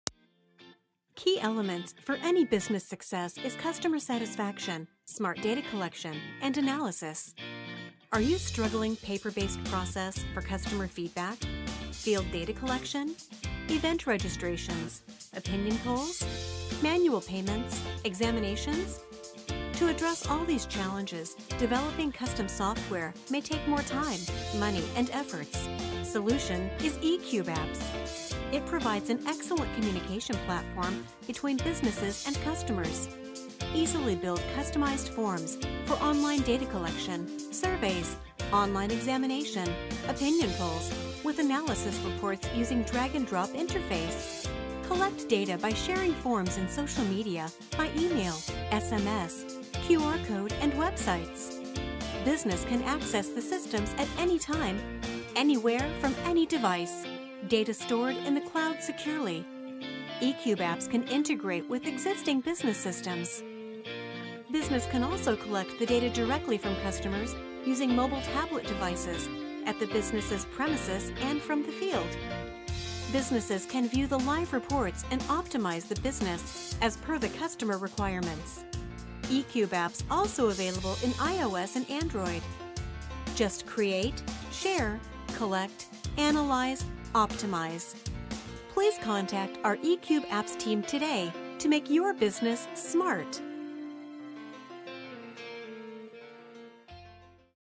eCube Apps : online form builder and online reporting app
eCube Apps provides an excellent communication platform between business owner and customers.

1. Easy and Quick to build customized forms for online data collection, Surveys, Payment Collection, Opinion polls with analysis reports using drag and drop interface.

2. Business can collect data from different sources like social media, email, SMS, QR Code and Websites.

3. Data stored in the cloud securely, business can access the system any time and any ware from any device.

4. Provides customized forms and Reports with good look & feel in line with business branding and promotion.

5. eCubeApps can integrate with existing business systems.

6. eCubeApps mobile application also available in IOS and Android